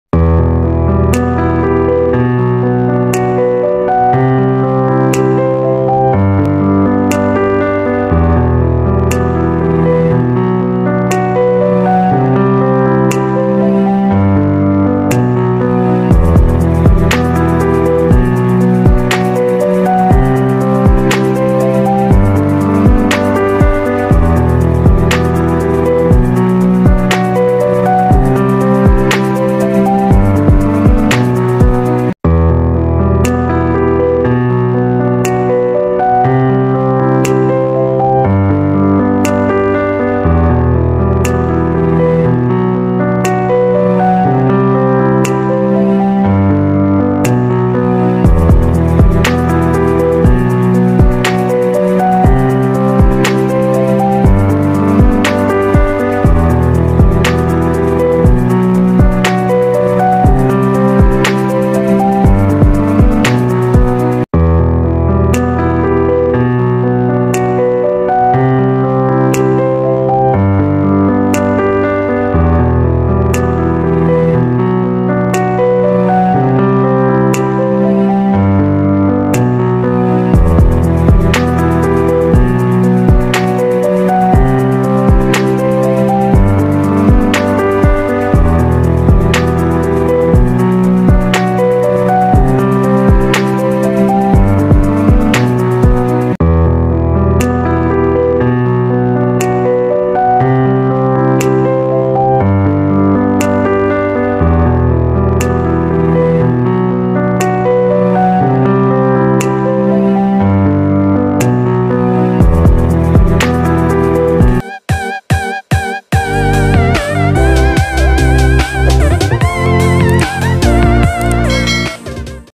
Two Minutes Instant Raqs E Bismil Hijab Tutorial by Sam Beauty and Crafts
In this Video I Am Gonna Tell You That How you can wear Hijab in two minutes